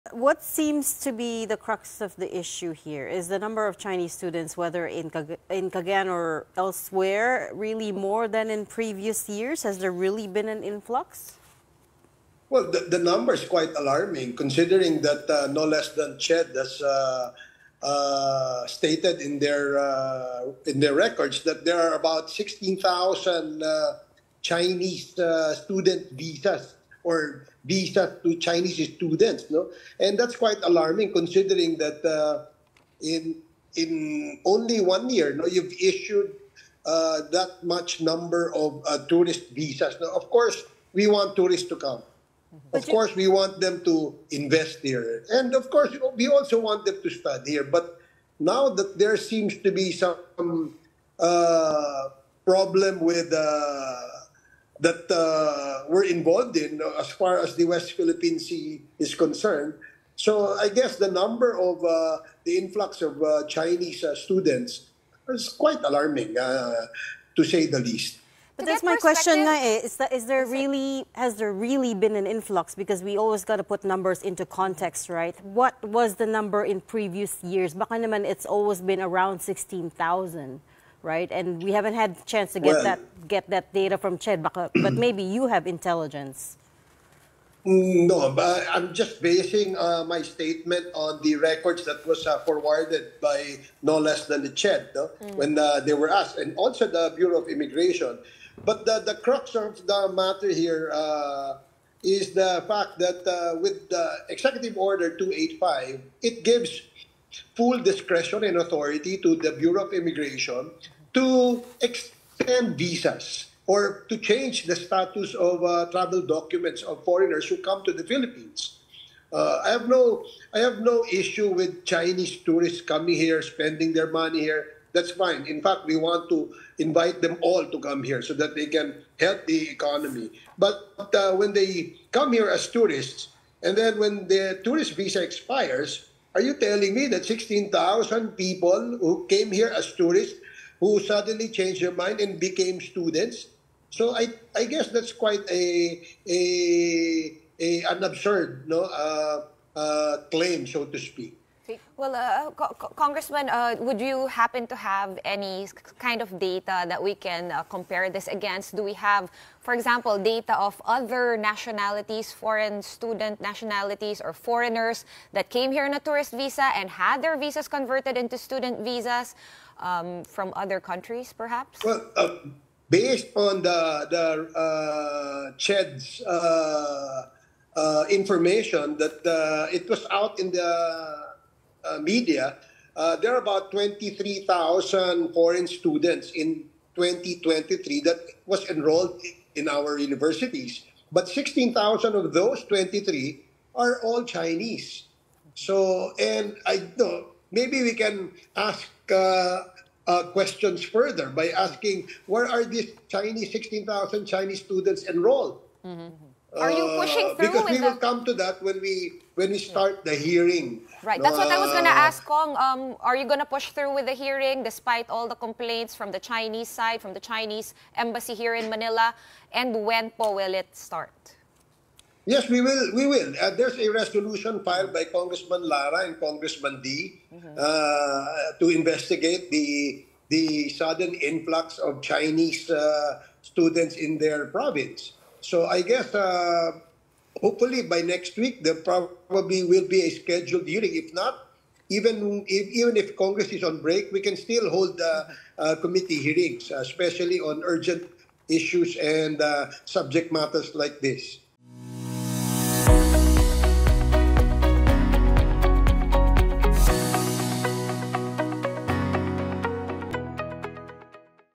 Rep. Barbers alarmed by “influx” of Chinese students in PH
TheBigStory | Surigao del Norte 2nd District Rep. Robert Ace Barbers wants a hearing on the “influx” of Chinese students, but also couldn’t say how many there were in previous years and whether the number has actually gone up.